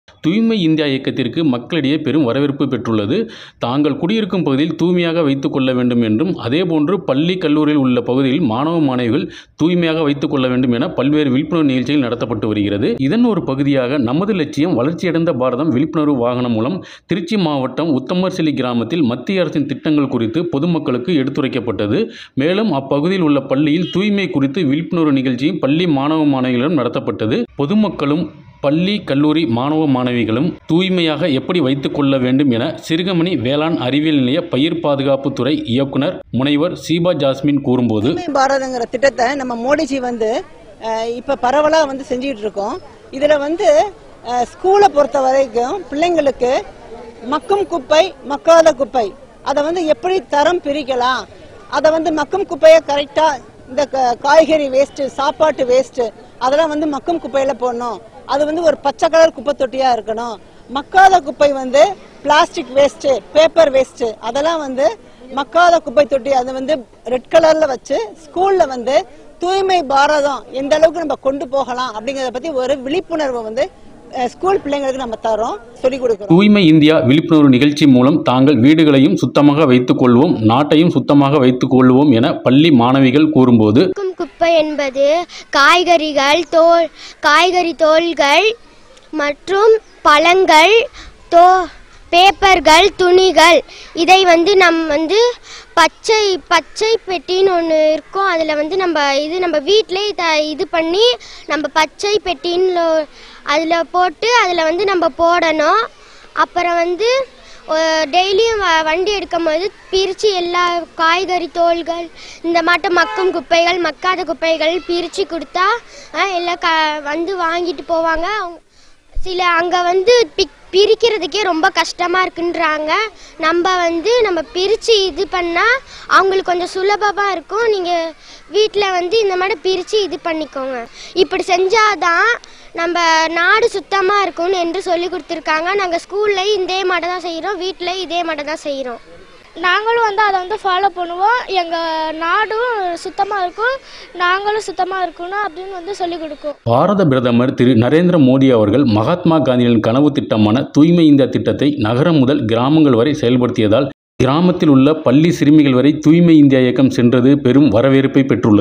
திருச்சி மாவட்டத்தில் மத்திய அரசின் நமது லட்சியம் வளர்ச்சி அடைந்த பாரதம் யாத்திரை நடைபெற்றது
திருச்சியில்  நமது லட்சியம் வளர்ச்சி அடைந்த பாரதம் விழிப்புணர்வு வாகனம் மூலம் தூய்மை இந்தியா திட்டம் குறித்து பள்ளி மாணவ, மாணவிகள் இடையே விழிப்புணர்வு ஏற்படுத்தப்பட்டது